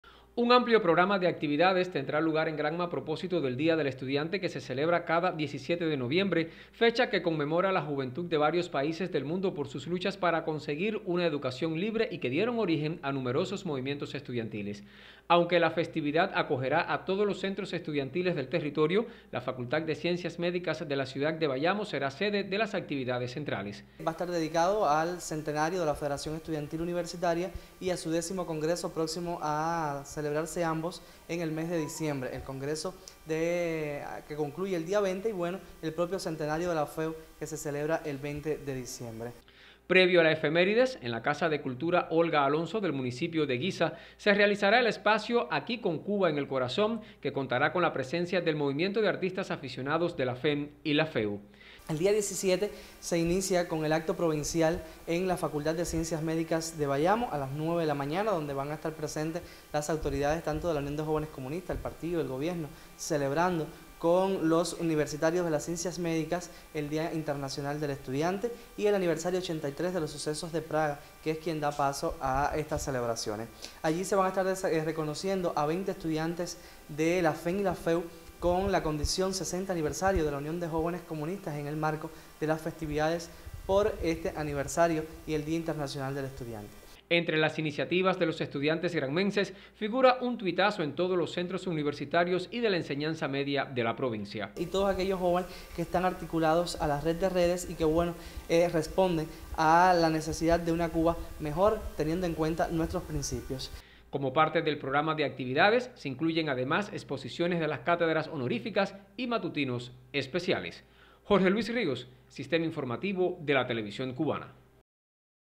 Desarrollarán amplio programa de actividades por el Día del Estudiante
Diversas actividades acontecerán en todos los centros estudiantiles de la provincia con motivo del Día Internacional del Estudiante, que se celebra cada 17 de noviembre. En el marco de la jornada serán reconocidos quienes se destacan en su quehacer docente y extracurricular.